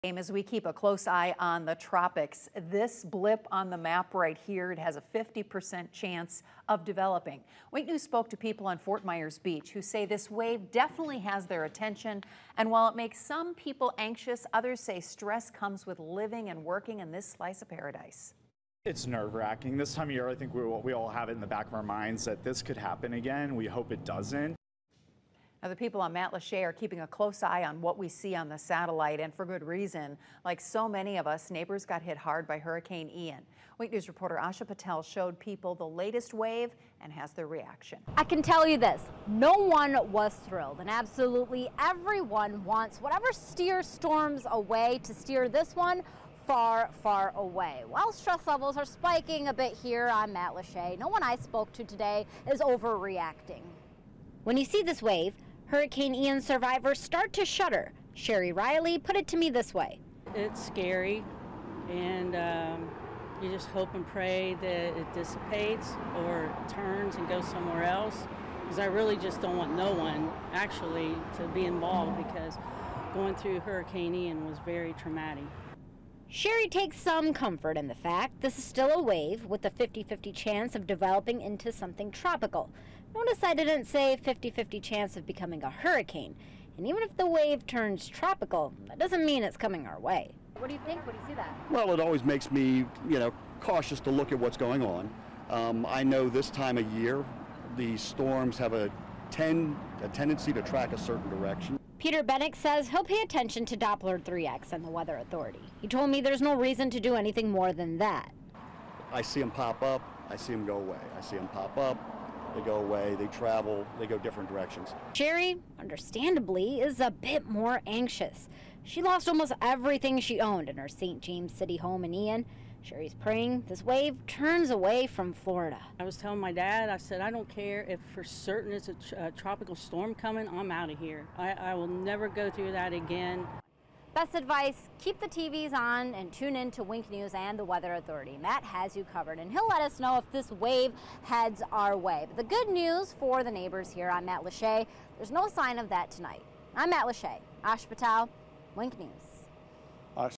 SWFL remains hopeful yet cautious in the face of potential tropical system
With peak hurricane season on the horizon and activity bubbling in the Atlantic, the wave we see on the satellite has southwest Florida's full attention.